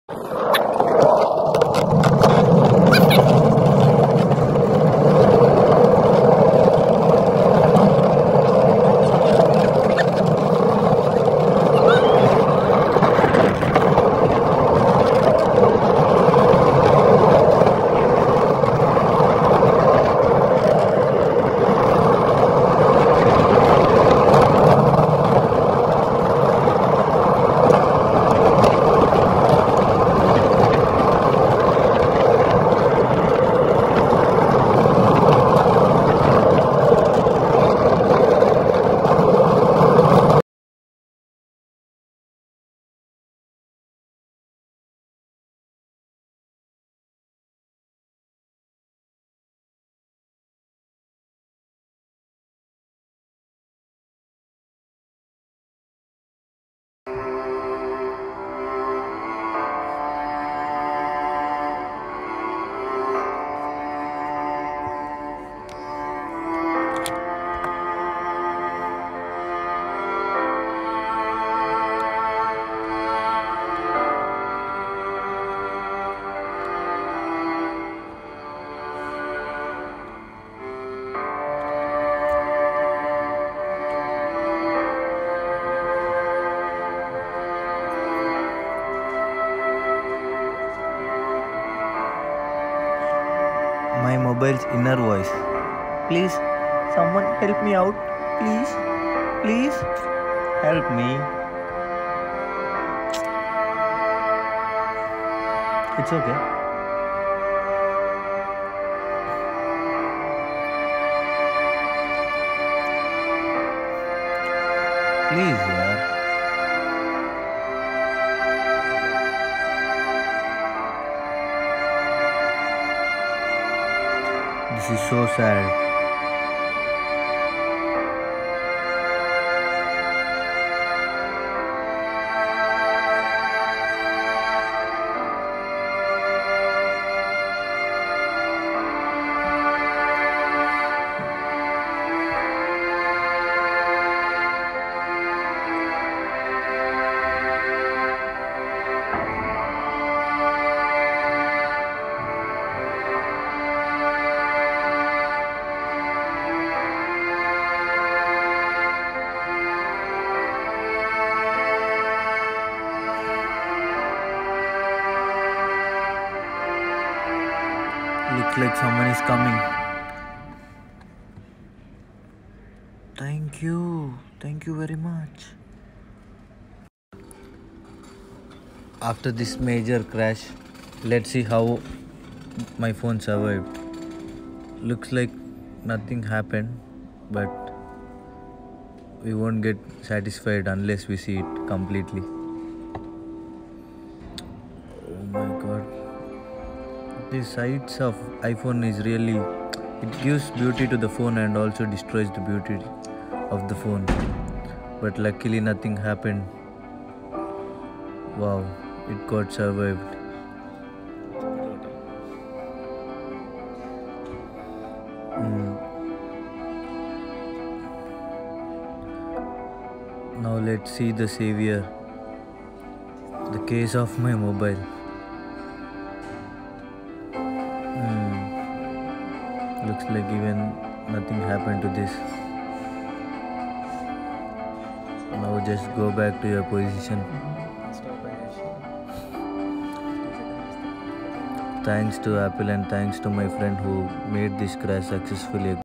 iPhoneXS Max crash :( //HD //
While recording a video my iPhone x s max fell down.